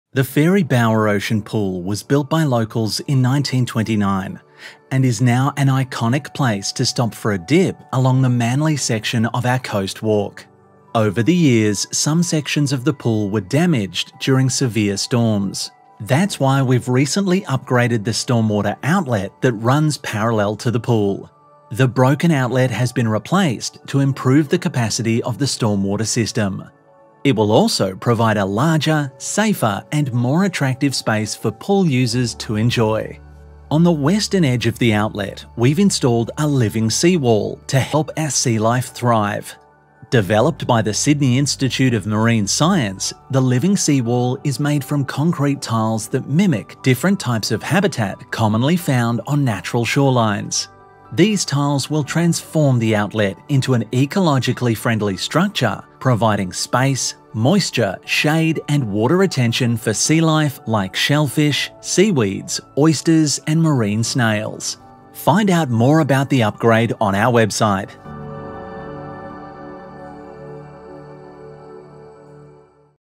Fairy Bower Ocean Pool has been renovated
We’ve recently upgraded the stormwater outlet at Manly’s iconic Fairy Bower Pool and created a larger space for pool users to sit, relax and enjoy the water view. We’ve also installed a Living Seawall to encourage more sea life to Cabbage Tree Bay Aquatic Reserve.